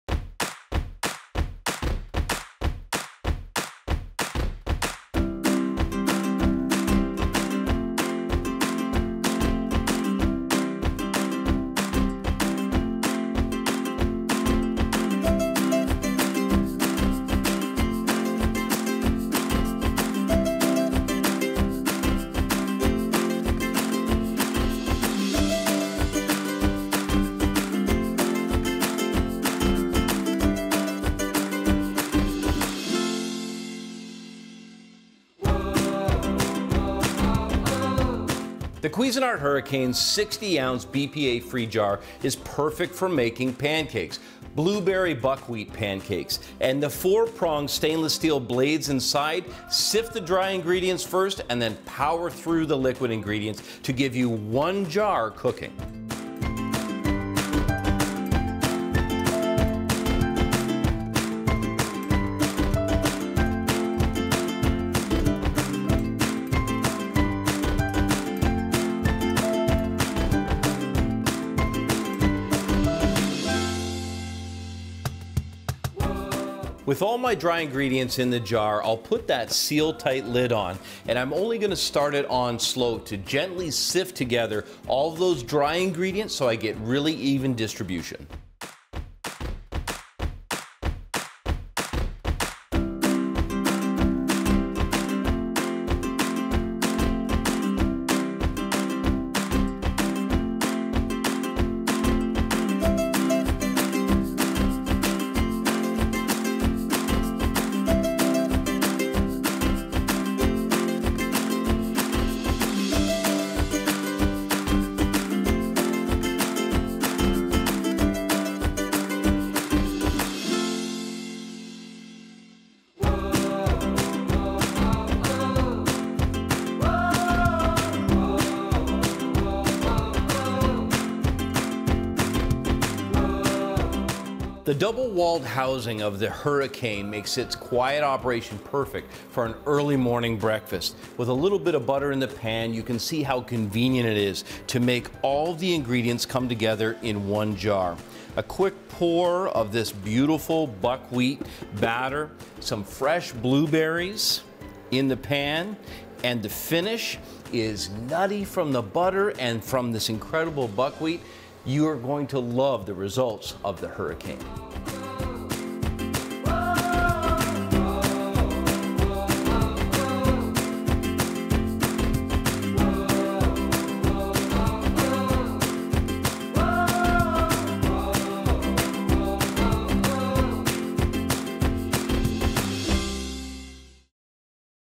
Blueberry Buckwheat Pancakes made with the Cuisinart® Hurricane™ Blender (CBT-1500C)
This Cuisinart® Hurricane™ blender is revved and ready to go. The professional power 2.25 peak HP motor handles even heavy pulverizing tasks with ease, and the double housing design keeps operation quiet. The 60-ounce (1.8L) BPA-free Tritan™ jar sits in a heavy duty plastic base and is uniquely designed for easy pouring, with a soft-touch handle for a secure grip. This blender operates at the touch of a button, with Low and High speeds, Pulse and preprogrammed Smoothie and Ice Crush options.